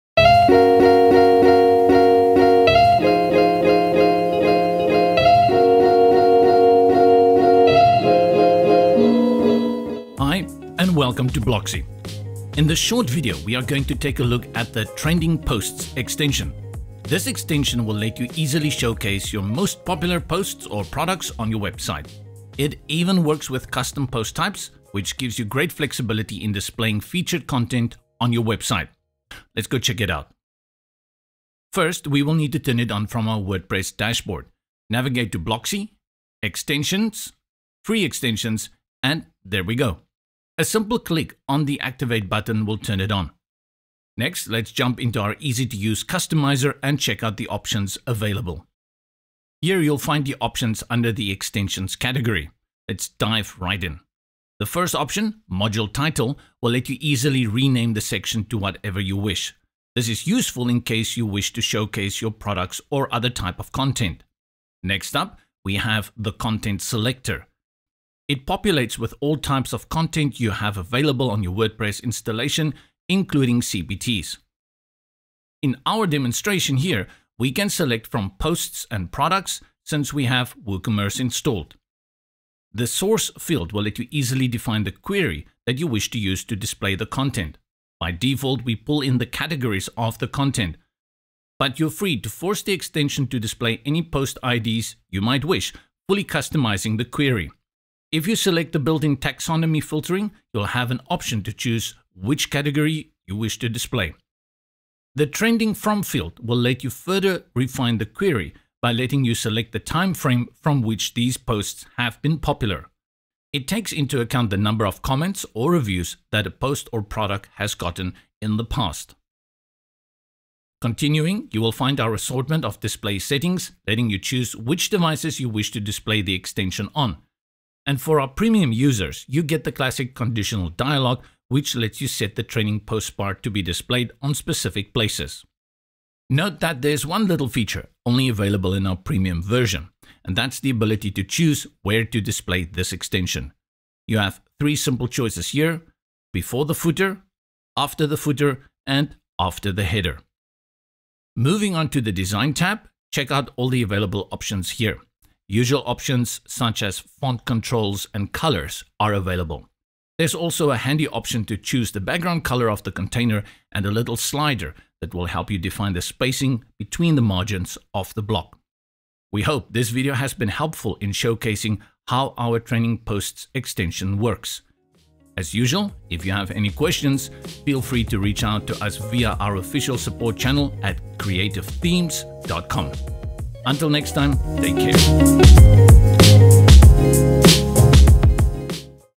Exploring the Trending Posts Extension | Blocksy Customisation
In this short video, we are going to take a look at the Trending Posts extension. This extension will let you easily showcase your most popular posts or products on your website. It even works with Custom Post Types, which gives you great flexibility in displaying featured content on your website.